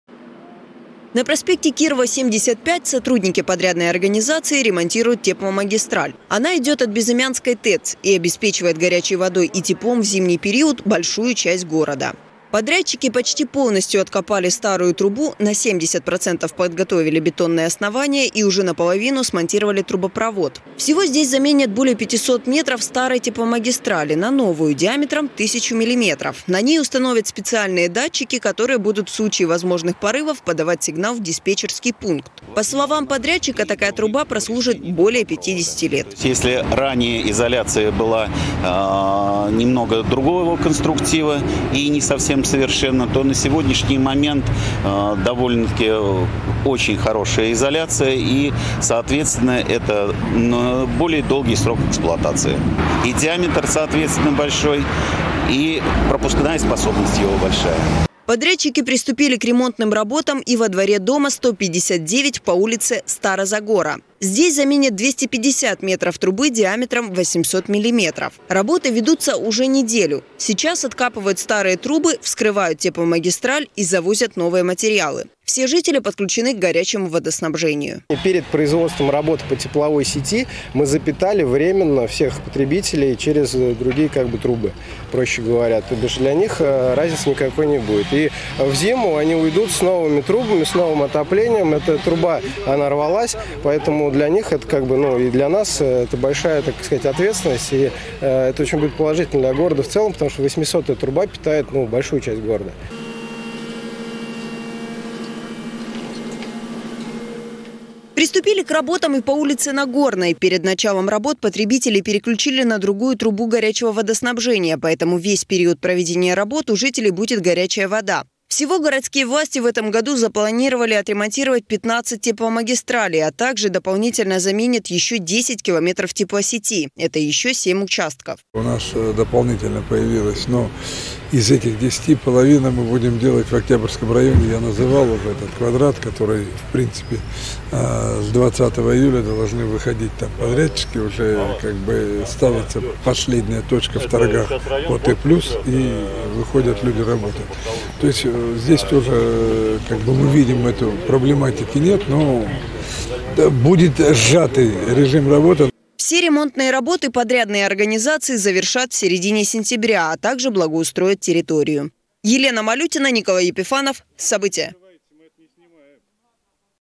В Самаре ремонтируют тепломагистрали. Останутся ли жители без горячей воды
В Самаре продолжается подготовка к отопительному сезону. Городские власти запланировали отремонтировать 15 тепломагистралей — на всех участках уже ведутся работы. Первый заместитель главы Самары Владимир Василенко провел выездное совещание. Вместе с ним на объекты отправилась Елена Малютина